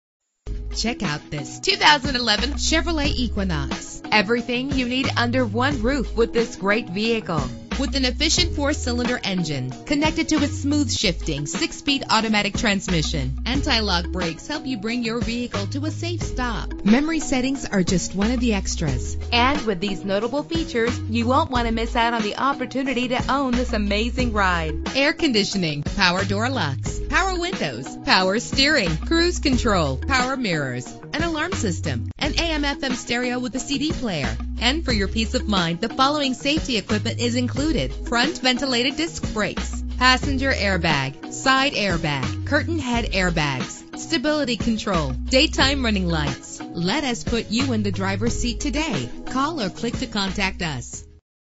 2011 Chevrolet Equinox - TARENTUM PA
Year: 2011
Make: Chevrolet
Model: Equinox
Trim: 1LT
Engine: 2.4 liter inline 4 cylinder 16 valve
Transmission: 6-Speed Automatic
Color: Supersonic Blue Metallic
Mileage: 20
Address:
22 WEST SEVENTH AVENUE
TARENTUM, PA 15084